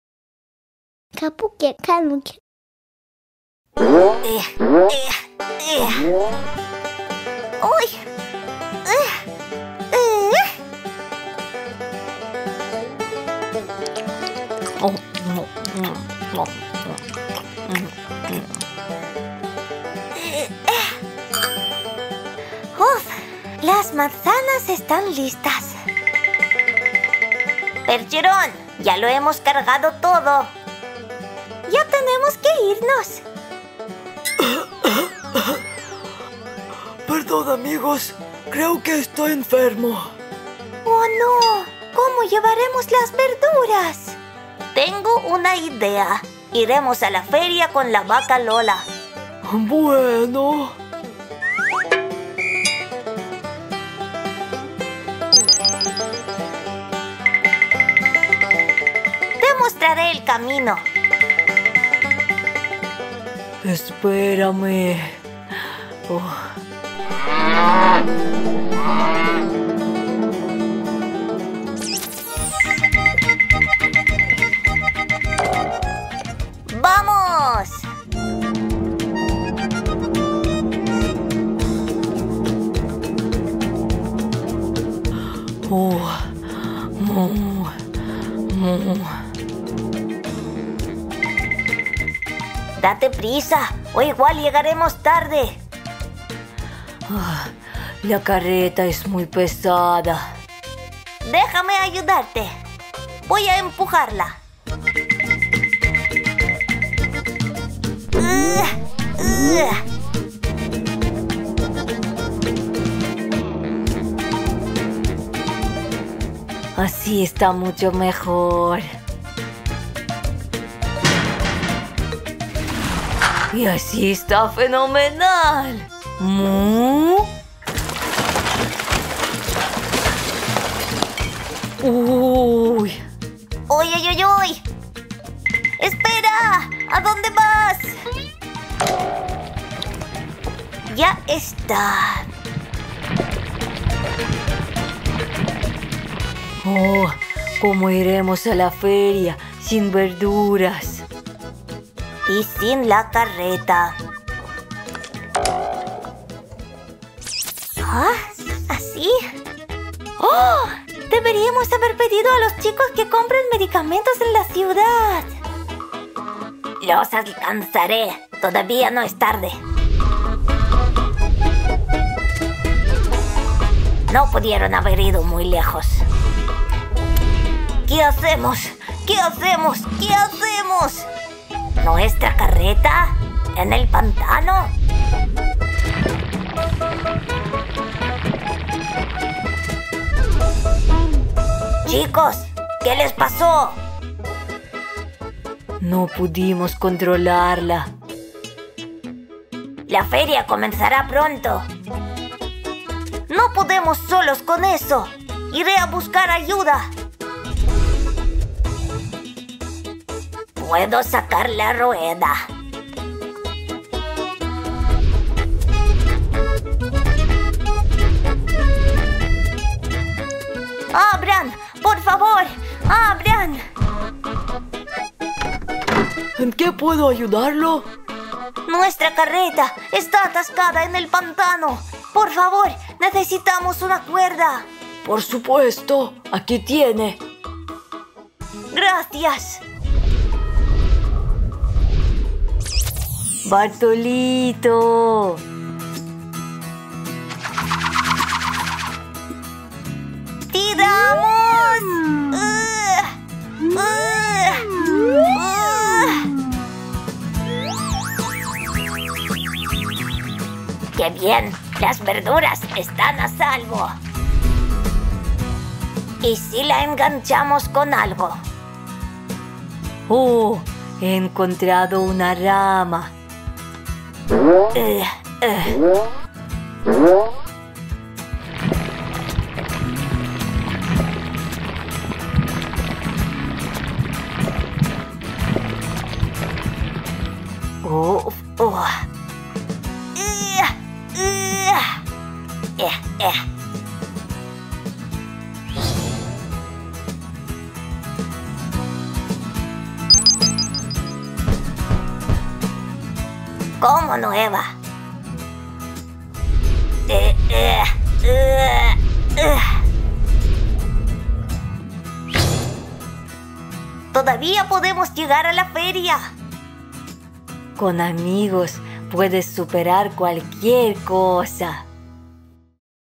¡El carrito de verduras de la Granja de Zenón está atascado! Video de juguetes para niños
Nuevo video de juguetes para niños con los animales de la Granja de Zenón en Kapuki Kanuki español. Hoy los animales van a la feria a vender las verduras. El caballo Percherón está enfermo, por eso la vaca Lola va a ser responsable por llevar el carrito con verduras.